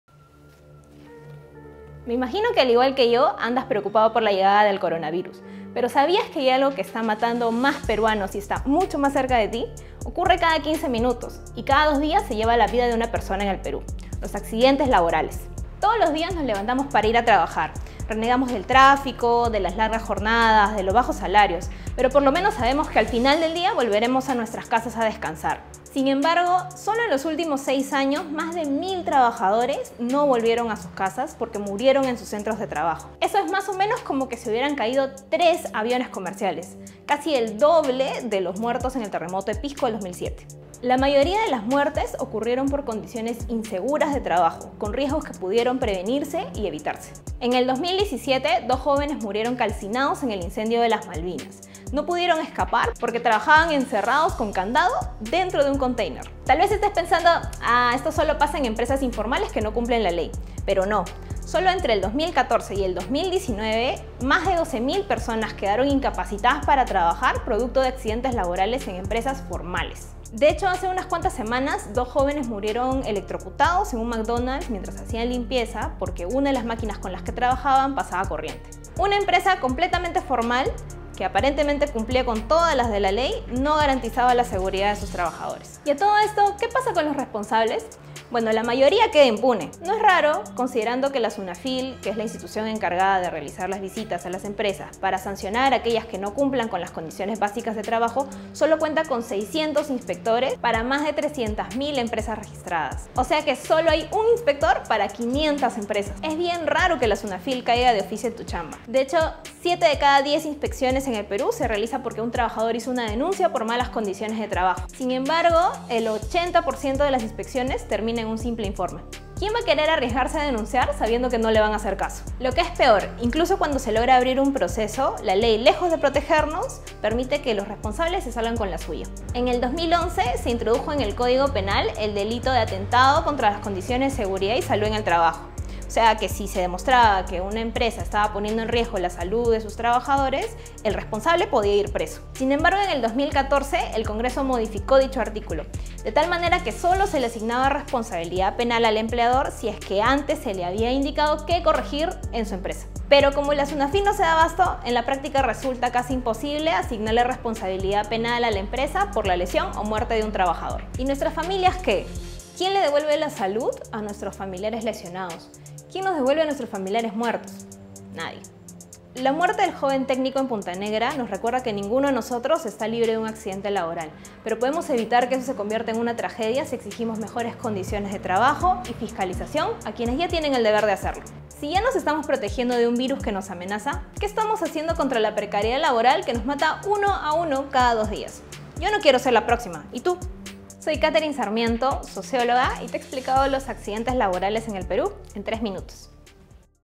¿Quién se hace cargo de los accidentes laborales?
Cada dos días muere una persona a causa de un accidente laboral.
¿Quién se hace cargo?

La socióloga Katherine Sarmiento te lo explica en 3Minutos⏱